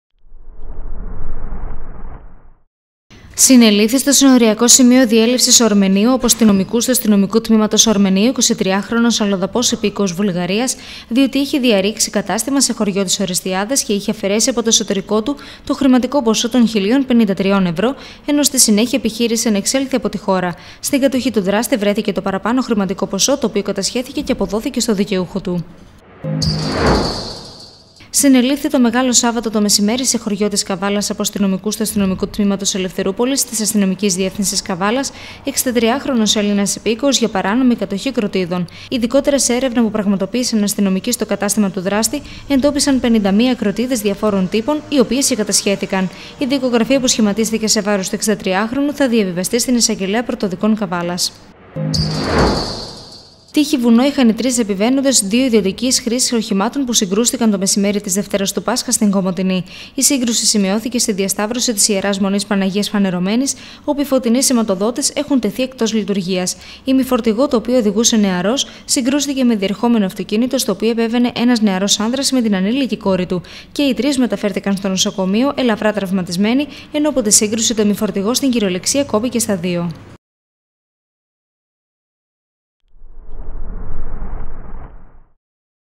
Συνελήφθη 23χρονος για κλοπή
Συνελήφθη, στο Συνοριακό Σημείο Διέλευσης Ορμενίου,  από αστυνομικούς του Αστυνομικού Τμήματος Ορμενίου, 23χρονος αλλοδαπός, υπήκοος Βουλγαρίας, διότι, είχε διαρρήξει κατάστημα, σε χωριό της Ορεστιάδας και είχε αφαιρέσει από το εσωτερικό του το χρηματικό ποσό των 1.053 ευρώ, ενώ στη συνέχεια επιχείρησε να εξέλθει από τη χώρα. Στην κατοχή του δράστη, βρέθηκε το παραπάνω χρηματικό ποσό, το οποίο κατασχέθηκε και αποδόθηκε στο δικαιούχο του.